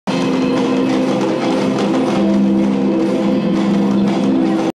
Maria Arena Bell at Yesssss! MOCA Gala 2013, Celebrating ...
Maria Arena Bell at Yesssss! MOCA Gala 2013, Celebrating The Opening Of The Exhibition Urs Fischer on 4/20/13 in Los Angeles, CA .